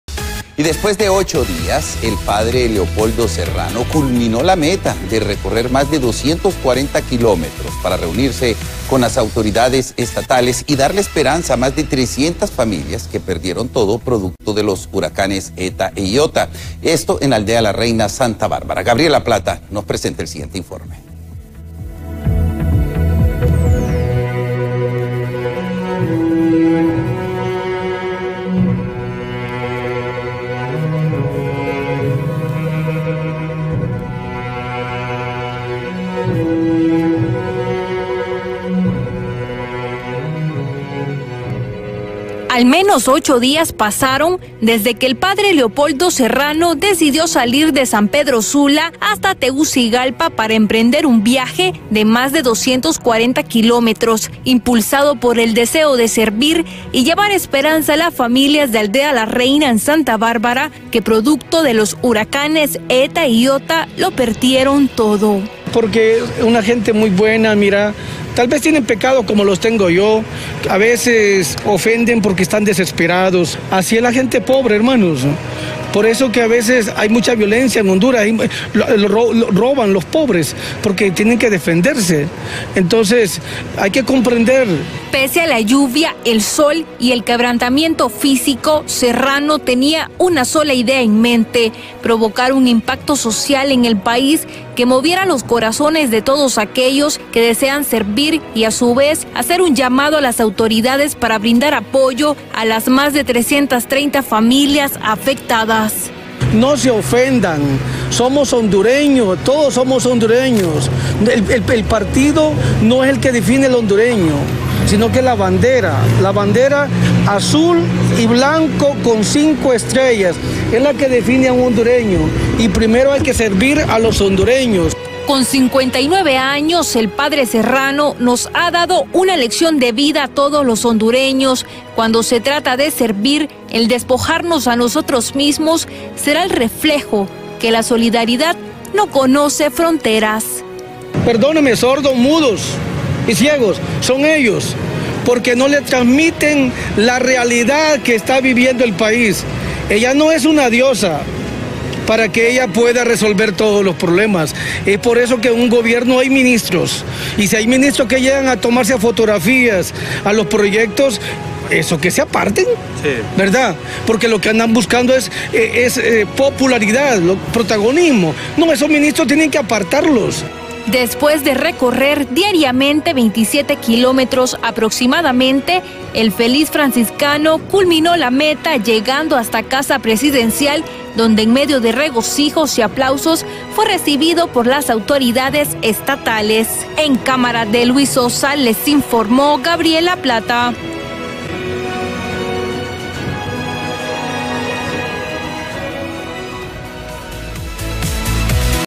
Padre Leopoldo Serrano culminó la meta de recorrer más 240 kilómetros
Le tomó al menos ocho días para darles esperanzas a varias familias.